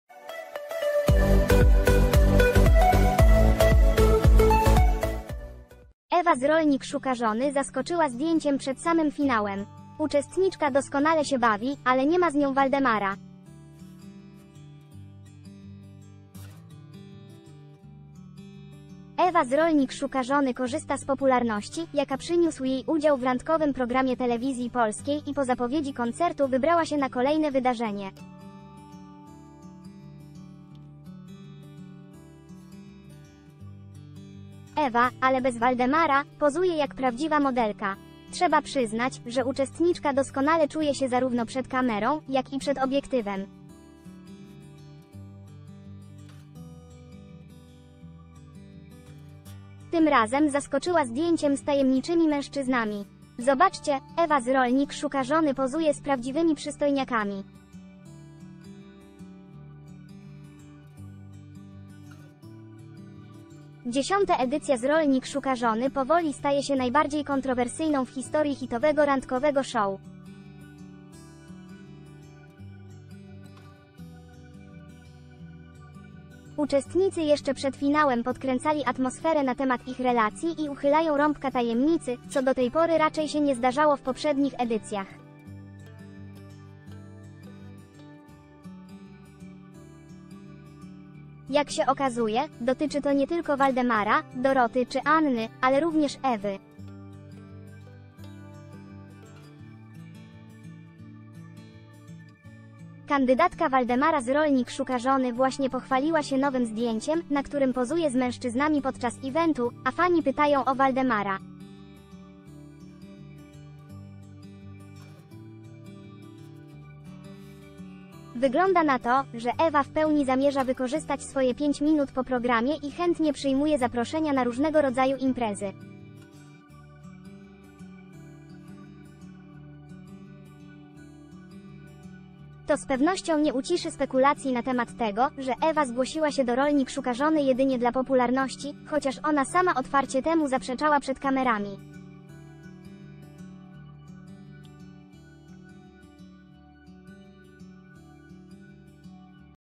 Ewa z "Rolnik szuka żony" pozuje z przystojniakiem. Fani: "Waldiego brakuje do tego disco"
Ewa z "Rolnik szuka żony" zaskoczyła zdjęciem przed samym finałem. Uczestniczka doskonale się bawi, ale nie ma z nią Waldemara!

Ewa z "Rolnik szuka żony" korzysta z popularności, jaka przyniósł jej udział w randkowym programie Telewizji Polskiej i po zapowiedzi koncertu wybrała się na kolejne wydarzenie. Ewa, ale bez Waldemara, pozuje jak prawdziwa modelka. Trzeba przyznać, że uczestniczka doskonale czuje się zarówno przed kamerą, jak i przed obiektywem. Tym razem zaskoczyła zdjęciem z tajemnic  ...